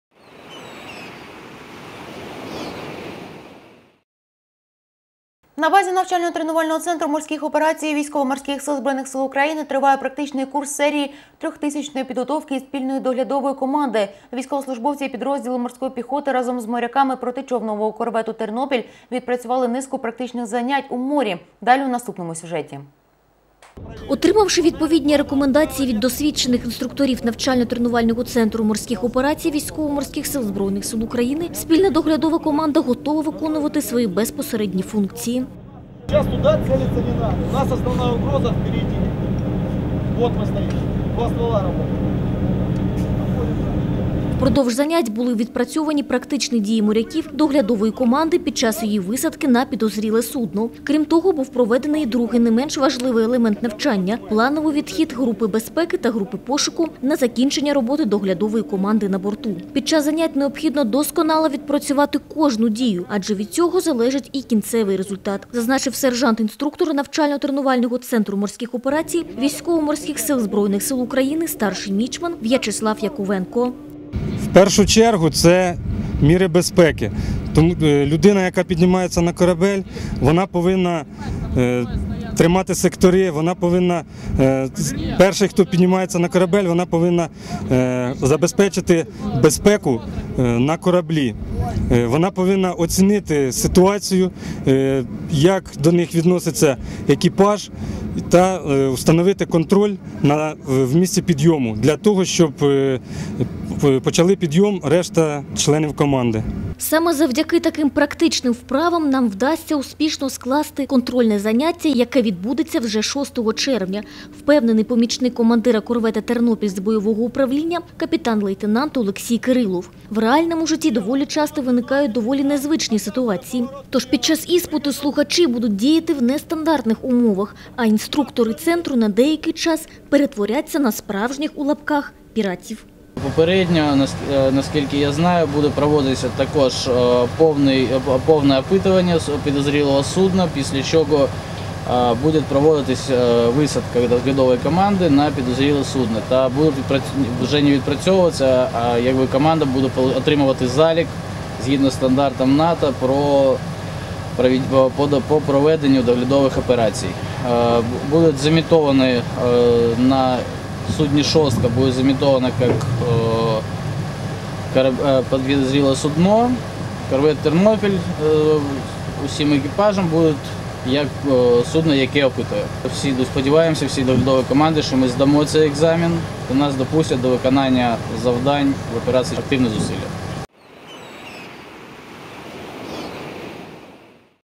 lЦМО ТЕРНОПІЛЬ
На базі навчально-тренувального центру морських операцій ВМС ЗС України триває практичний курс серії 3000 підготовки спільної доглядової команди. Військовослужбовці підрозділу морської піхоти разом із моряками протичовнового корвету «Тернопіль» відпрацювали низку практичних занять в морі.